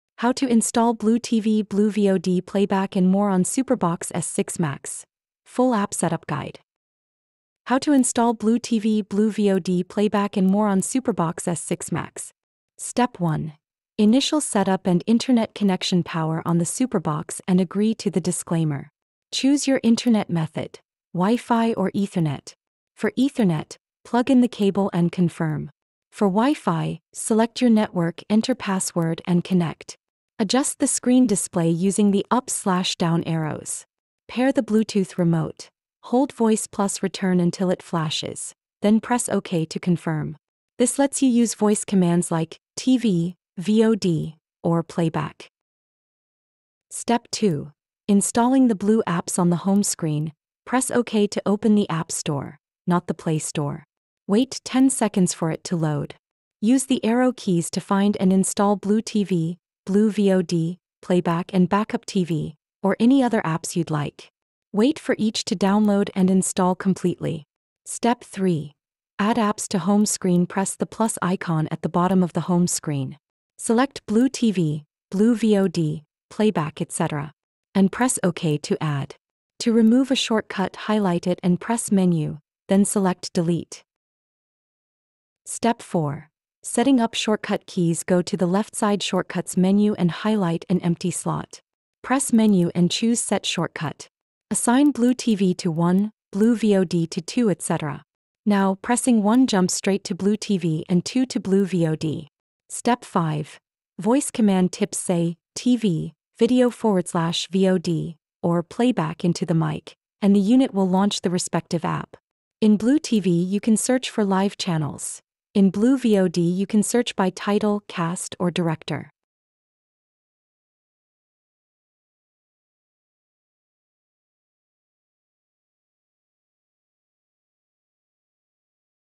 How To Install Blue TV, Blue VOD, Playback & More On SuperBox S6 Max – Full App Setup Guide
Updating your SuperBox S6 Max firmware is essential for optimal device performance. In this video, we will guide you through the entire update process, ensuring you have the latest version installed. Our step-by-step instructions are designed for users of all experience levels.